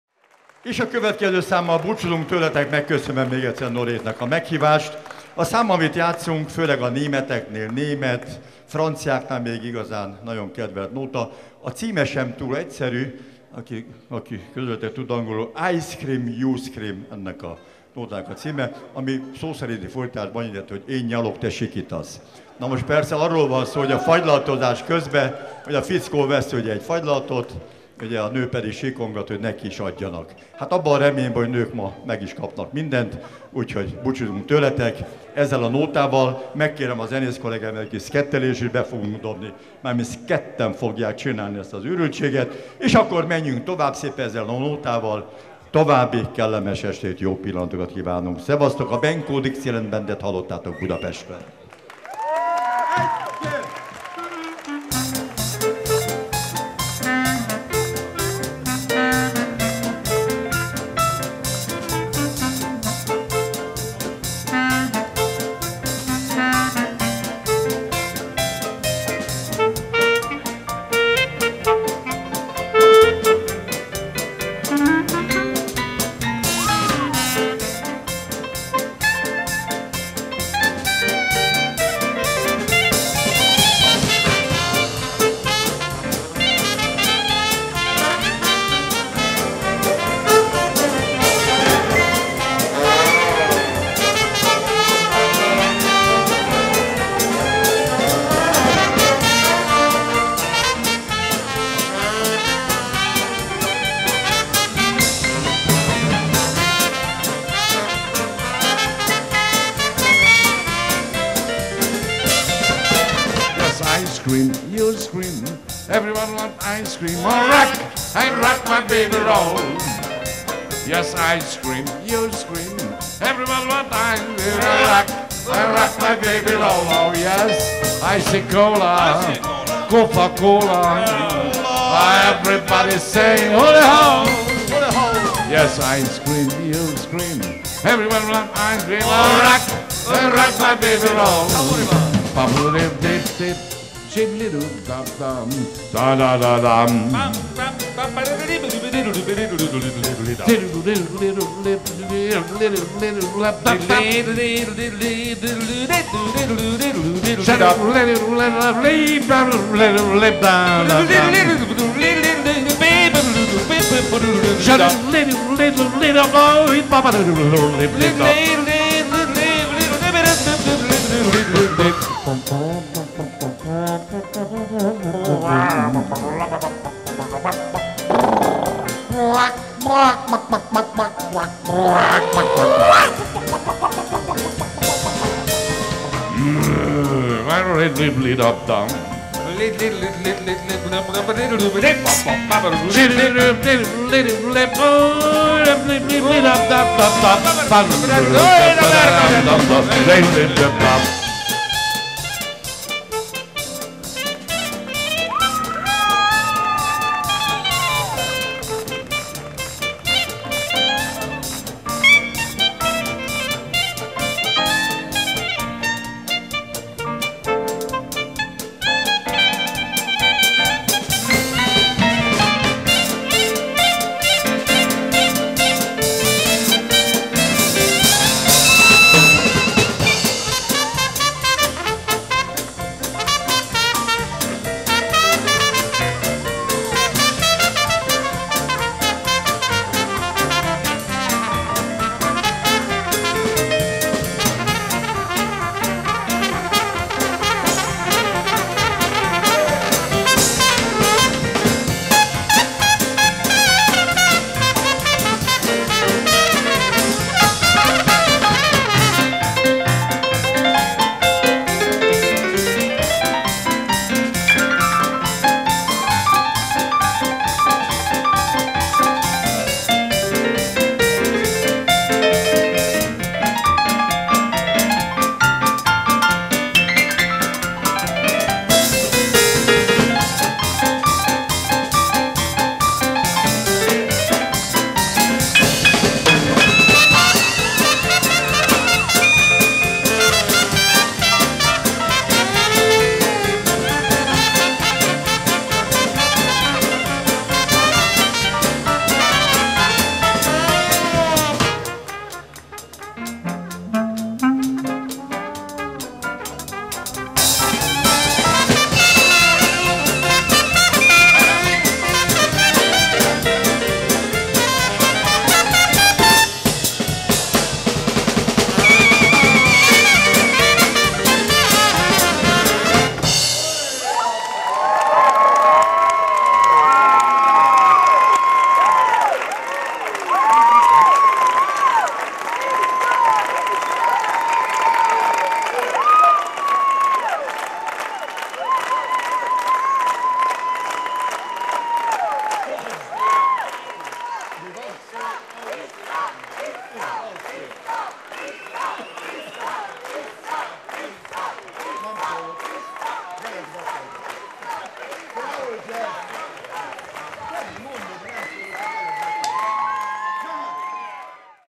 BENKÓ DIXIELAND BAND (HU) - III.MiniJazz Festival 2011.Csíkszereda / Miercurea Ciuc (4)
Benkó Sándor -- klarinét, zenekarvezető

Nagy Iván -- pozán, ének

Nagy Jenő -- bendzsó

Szalóky Béla -- trombita

Halmos Vilmos -- zongora, ének

Csikós Miklós -- bőgő

Kovacsevics Gábor -- dob